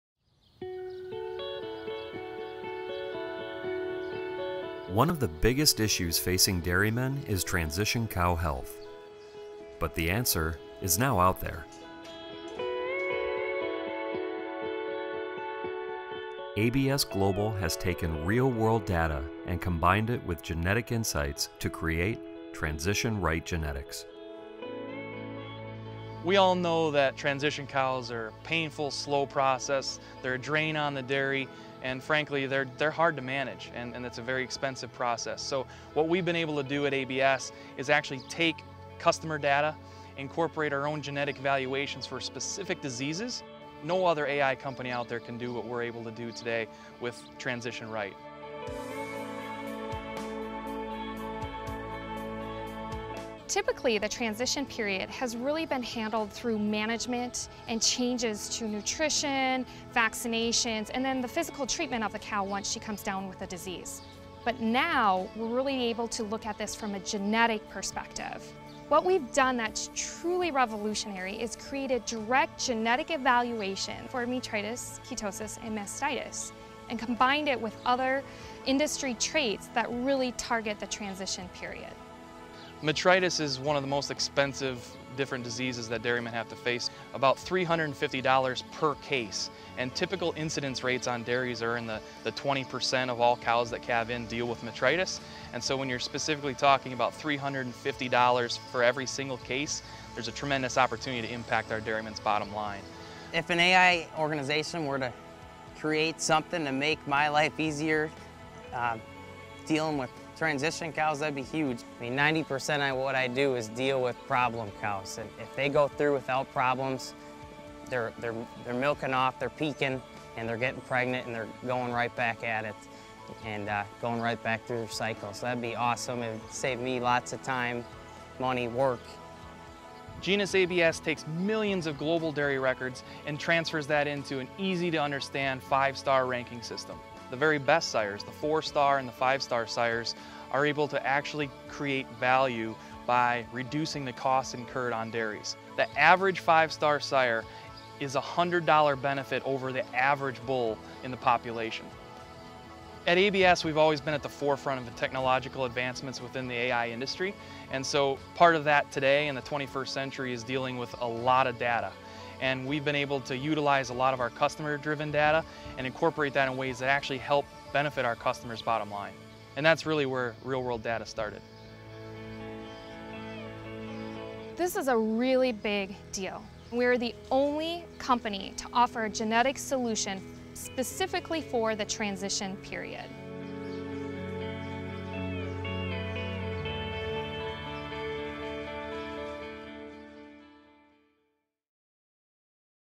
ABS TransitionRight | Genetic Solution to Prevent Transition Health Problems
Transition health disorders cost you serious time, money, productivity and cows. ABS’s TransitionRight offers you a genetic solution to proactively help prevent transition health problems in your herd, by making your cows more genetically predisposed to reduce disorders such as Mastitis, Metritis and Ketosis. To get ABS TransitionRight Genetics in Pakistan, contact Solve Agri Pak (Private) Limited, authorized ABS distributor in Pakistan,